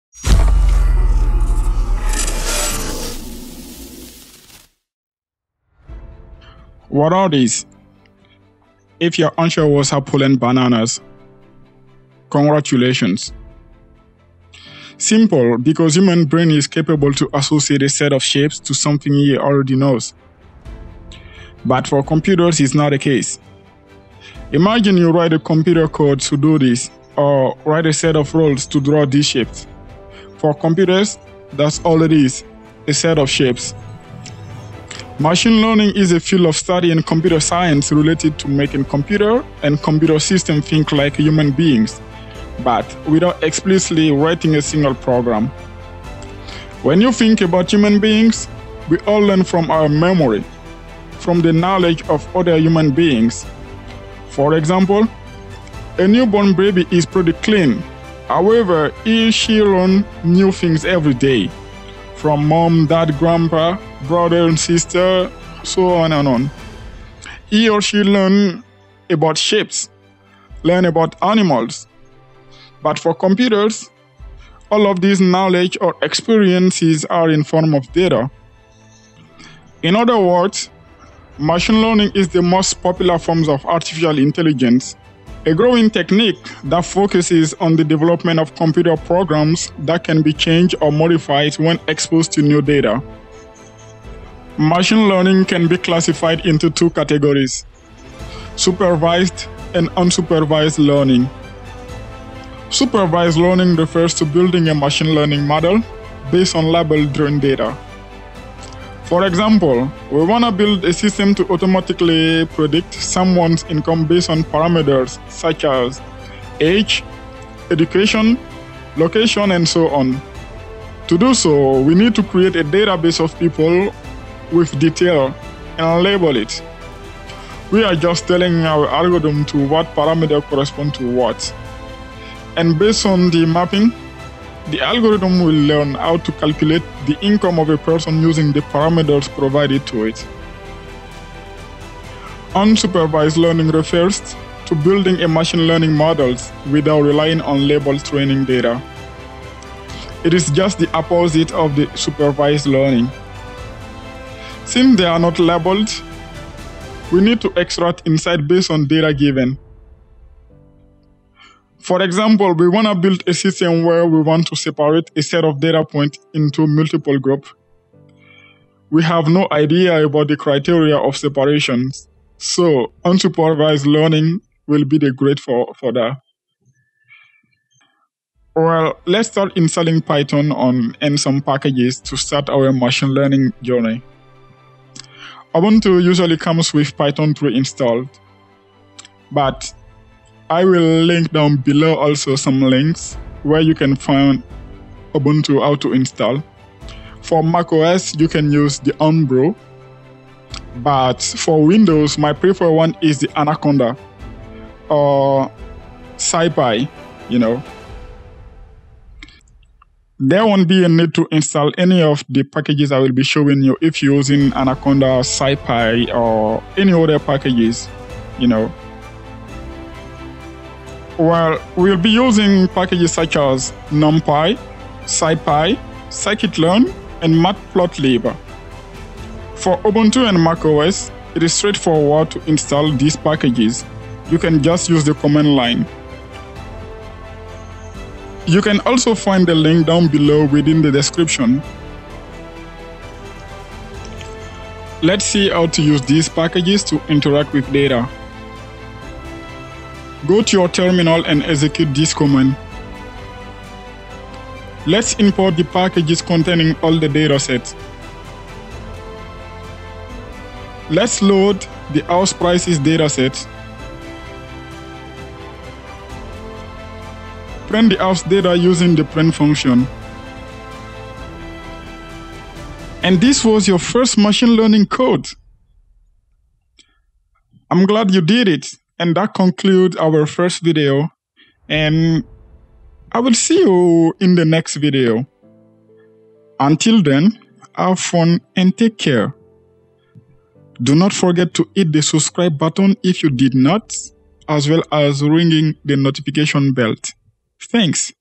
Machine Learning # 1 | Basics Supervised vs Unsupervised Learning
Machine learning is the most popular form of AI. Design and develop software that can learn from data. We perform predictions on unknown data based on learning. Trained to compare in form of pattern. 1 Like = 1 Chatbot